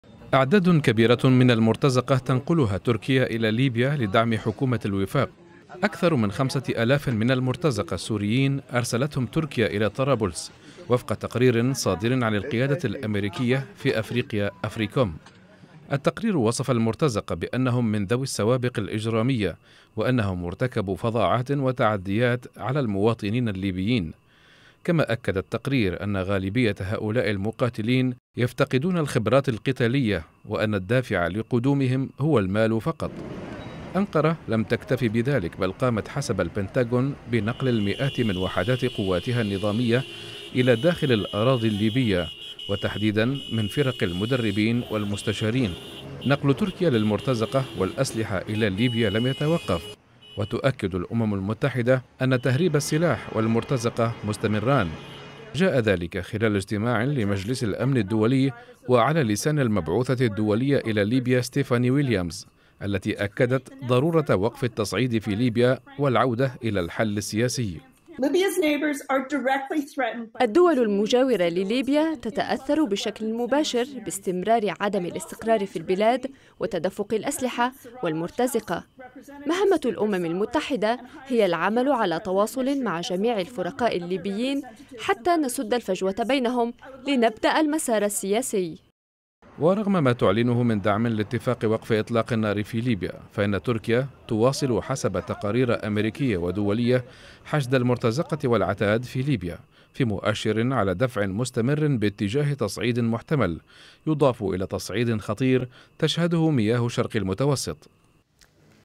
البنتاغون يؤكد إرسال تركيا نحو 5 آلاف مرتزق سوري إلي ليبيا
أكد تقرير صادر حديثًا عن وزارة الدفاع الأميركية البنتاغون أن تركيا أرسلت أكثر من 5 آلاف عنصر من المرتزقة السوريين لدعم ما تسمى بحكومة الوفاق الوطني في طرابلس، ضد الجيش الوطني الليبي. وقال البنتاجون، إن القيادة الأمريكية في أفريقيا "أفريكوم" رصدت وصول مرتزقة سوريين بأعداد كبيرة إلى ليبيا على متن طائرة عسكرية تركية.. المزيد من التفاصيل عبر التقرير المصور.